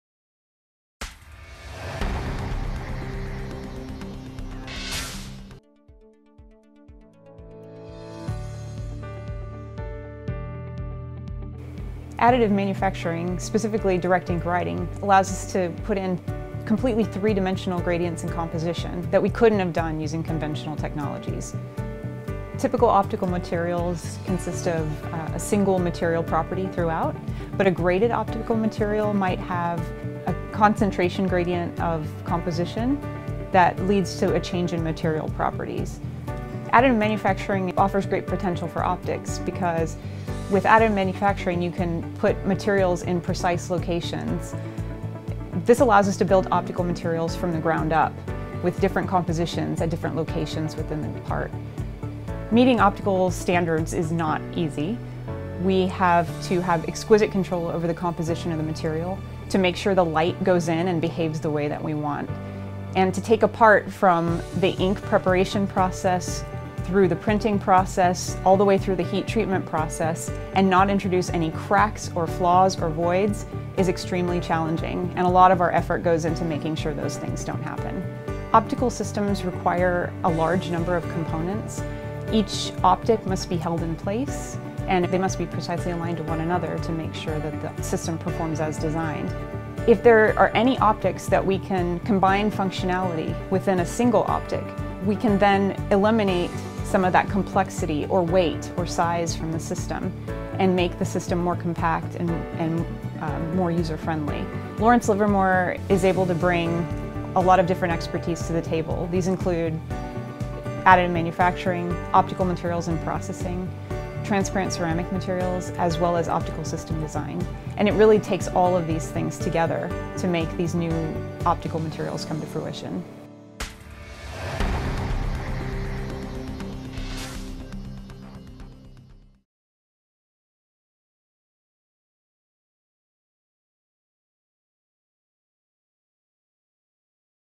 The Dawn of an Optical Revolution | S&TR Preview
Combining optics design and additive manufacturing technology, Livermore creates a new class of functionally graded optical materials.

Read more about it in this month's issue of Science & Technology Review (S&TR): str.llnl.gov.